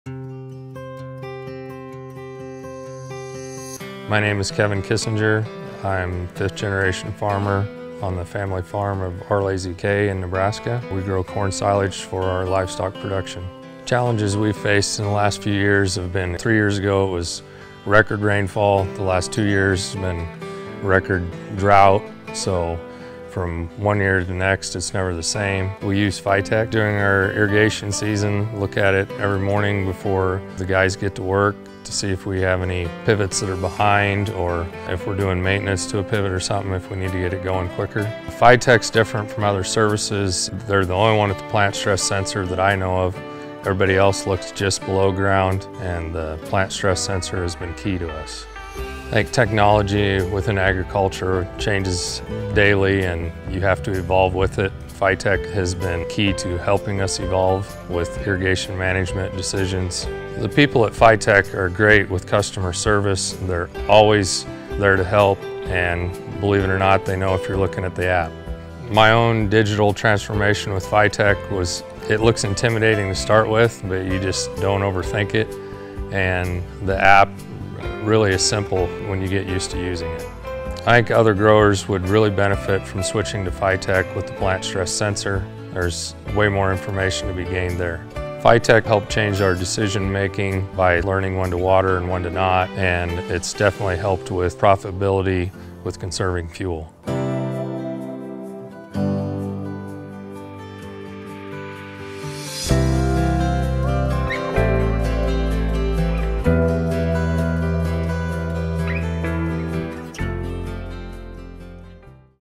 Grower Testimonial: Kevin Kissinger / R Lazy K Farm
In our latest Customer Testimonial, Kevin Kissinger, Owner of R Lazy K Farm, explains how Phytech and Nic Swanson have helped R Lazy K Farm navigate climate extremes in the last 3 years. Knowing when to irrigate based on plant stress data has been critical when navigating years with record rainfall or record drought.

“Phytech helped change our decision-making by learning when to water and when to not. Also, it’s definitely helped with our profitability by conserving fuel.” - Kevin Kissinger, Owner of R Lazy K Farm

Thank you, R Lazy K Farm. We at Phytech are proud to help you optimize your irrigation strategy.